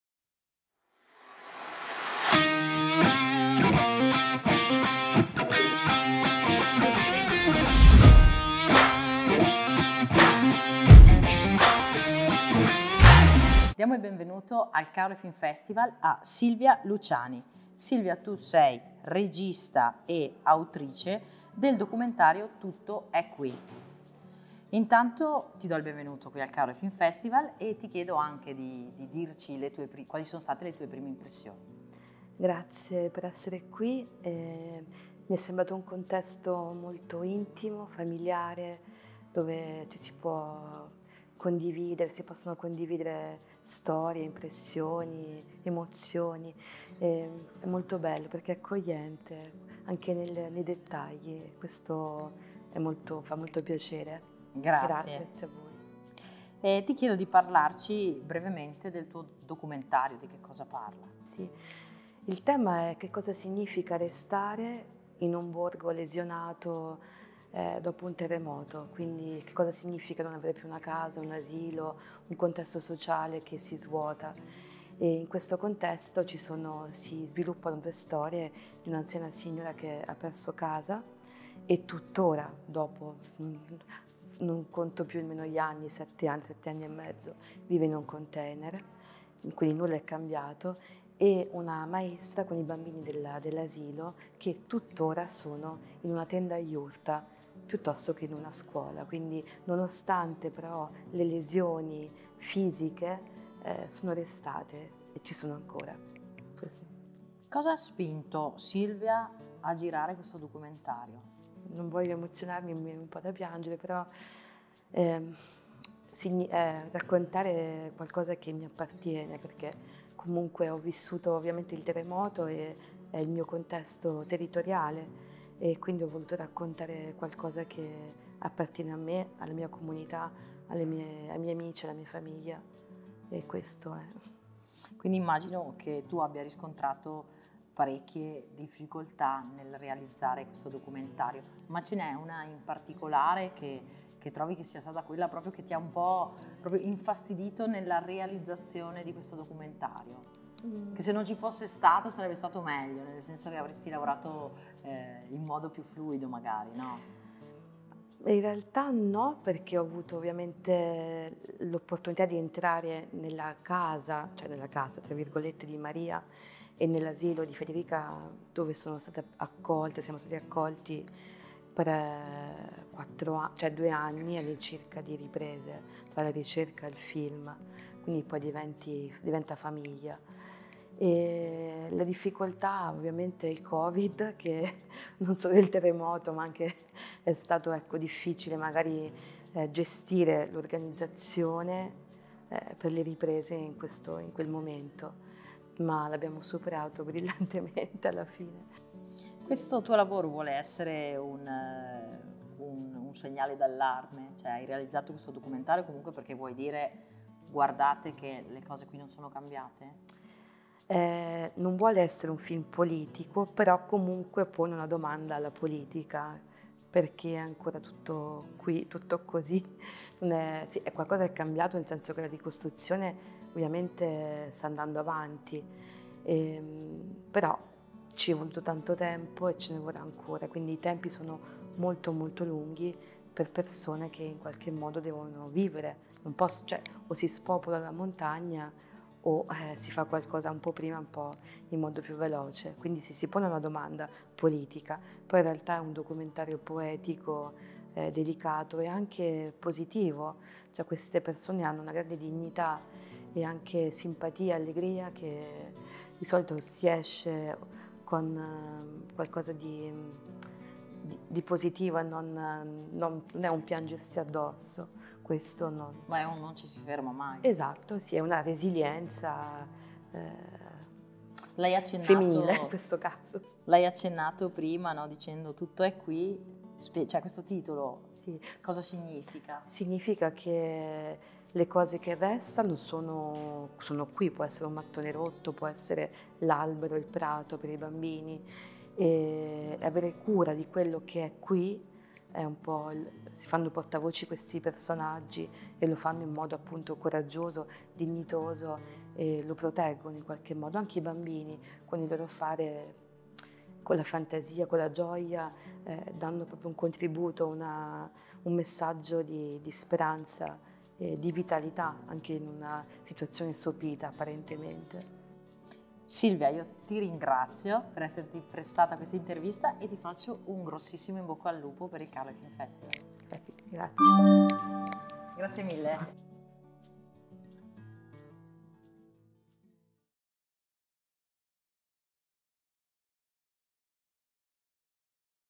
Intervista a Silvia Luciani | Caorle Independent Film Festival 2023
Intervista a Silvia Luciani, autore del documentario Tutto È Qui, in selezione ufficiale della sesta edizione del Caorle Independent Film Festival 2023.

Intervista a cura di Valentina Corte di Radio Academy.
Riprese a cura di Pietro Marin & Maristella Gusso.
Montaggio a cura di Andrea Malpighi.